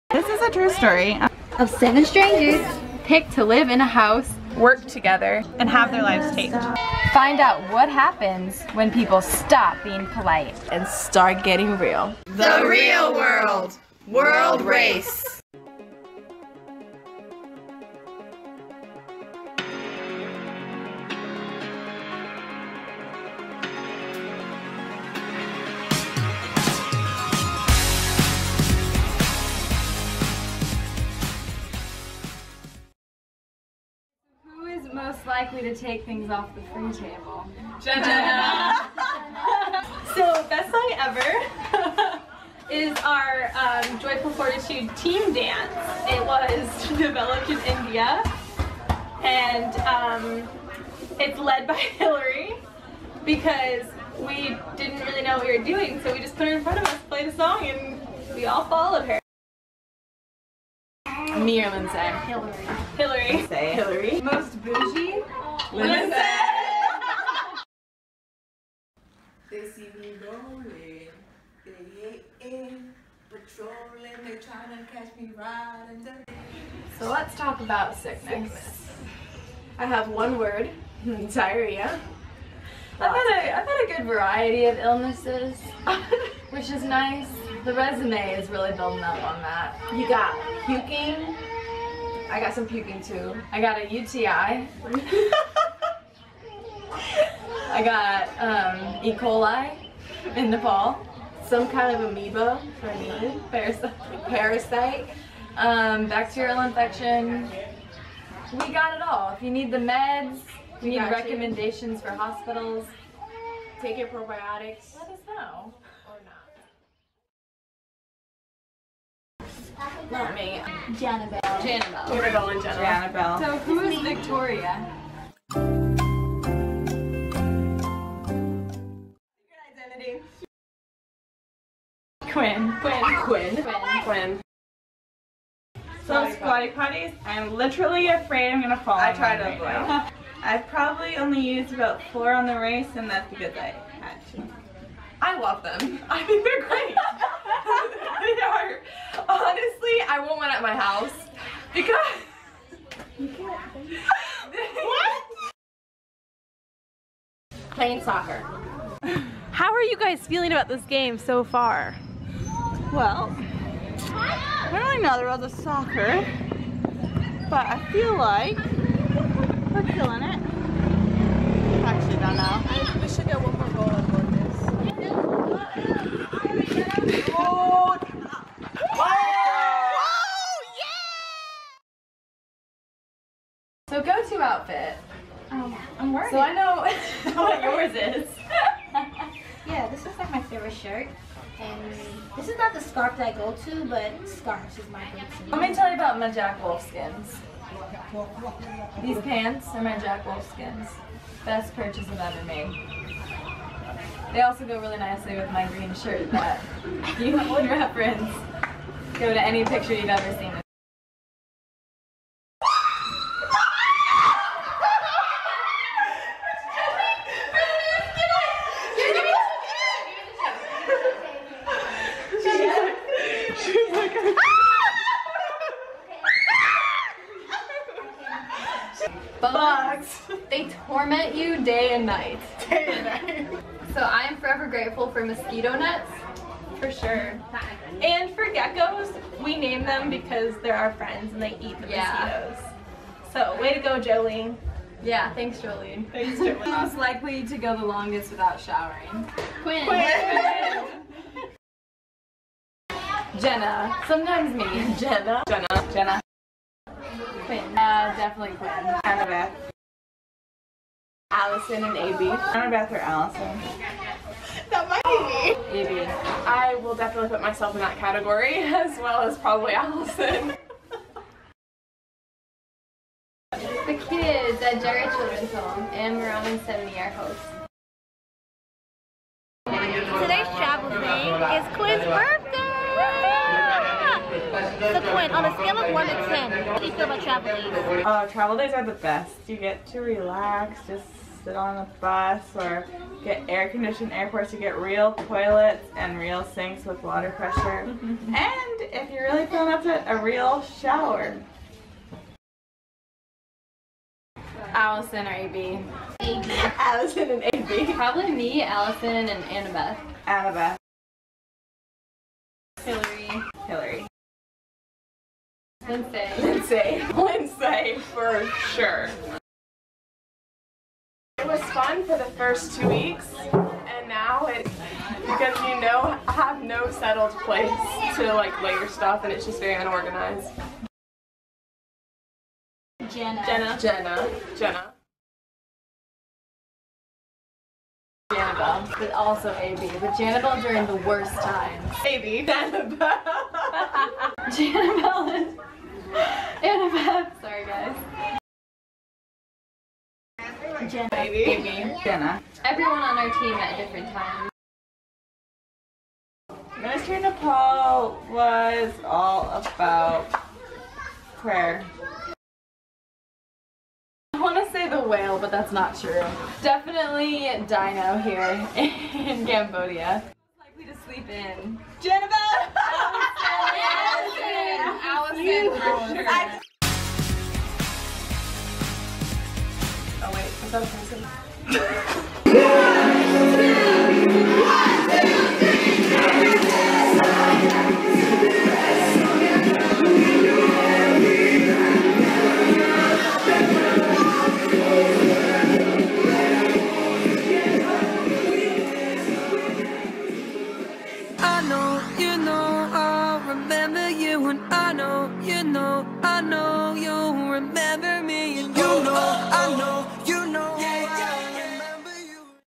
The Real World: World Race
For the last four month I had an opportunity to be in community with six other ladies.  This is our real world experiences ... doing ministry daily, traveling from country to country, sharing the gospel of Christ  fighting off bugs, sleeping in the most uncomfortable places but ultimately growing together and learning more about ourselves.